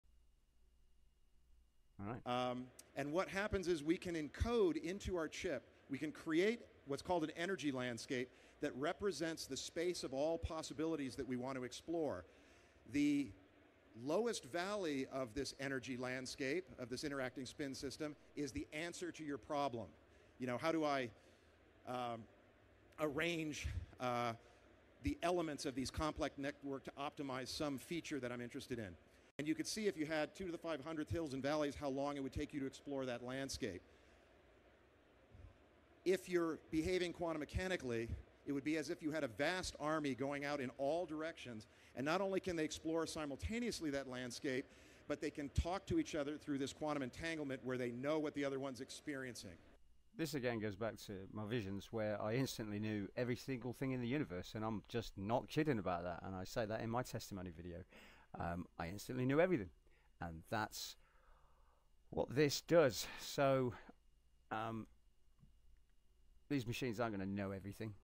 Unlocking Quantum Possibilities: Optimizing Complex Networks
We delve into how encoding energy landscapes in our chip helps solve complex problems. Explore how quantum mechanics enables simultaneous exploration of vast possibility spaces, revealing insights that transform our understanding of network optimization.